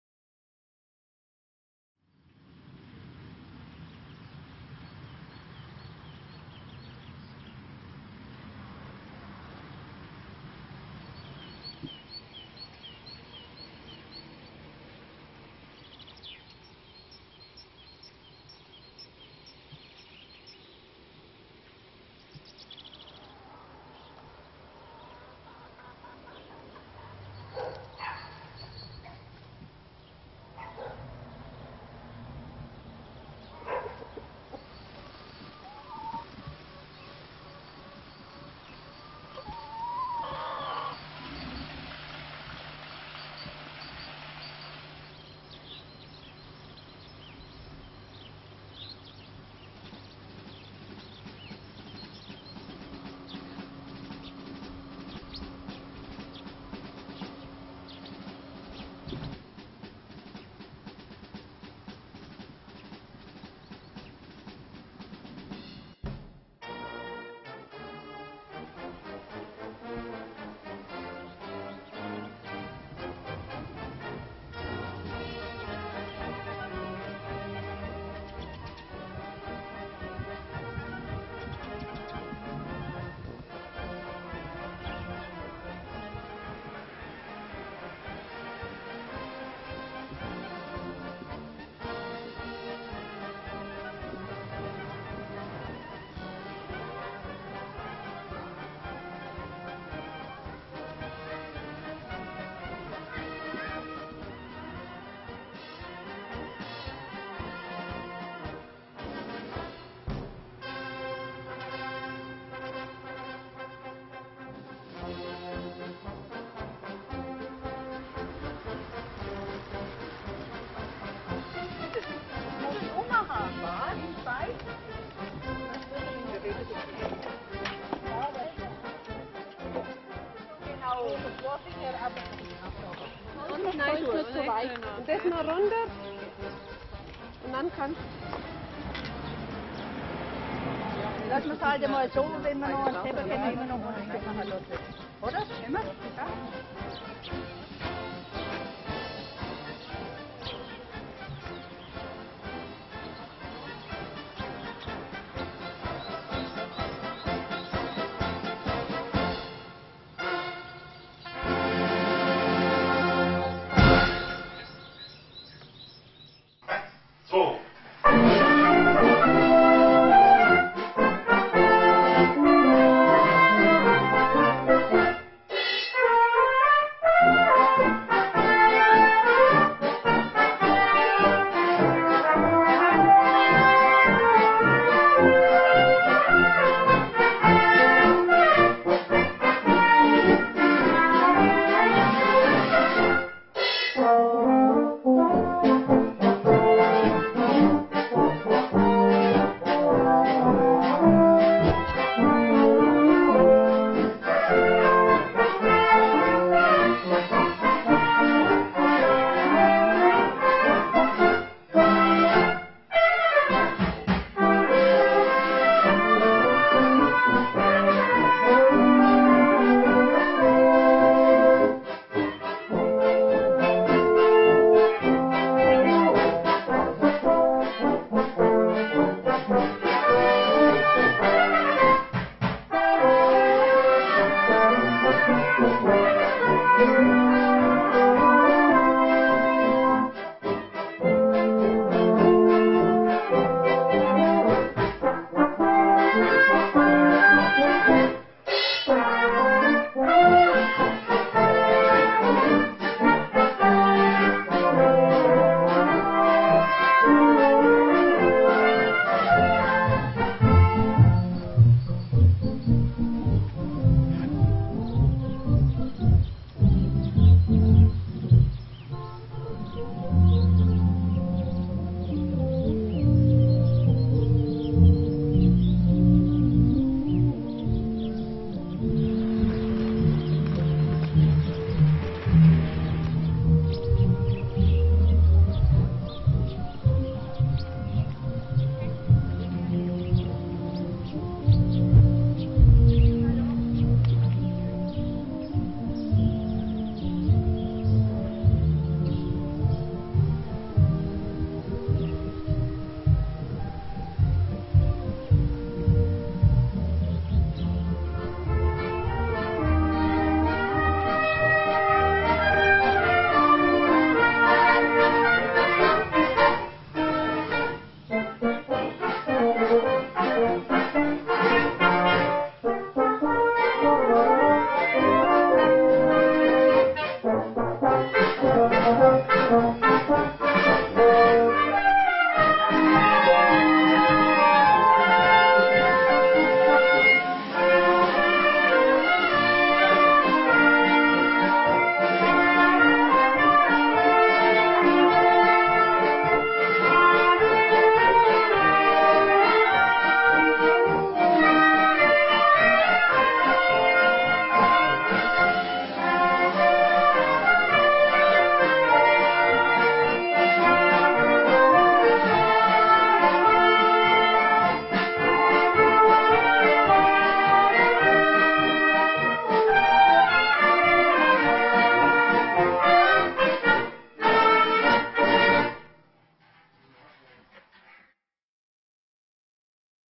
" Blasmusik Illenschwang" und das kleine Gemeinde Illenschwang in Bayern
Ein Videobericht von Illenschwang in Bayern, nahe Dinkelsbühl, wo wir auch die Blasmusik  Illenschwang bei  Übung begegnen. Ein Dorf in Bayern ist schön auch für ein Norweger! Dies ist Version 2, mit Copyright Meldungen eingefügt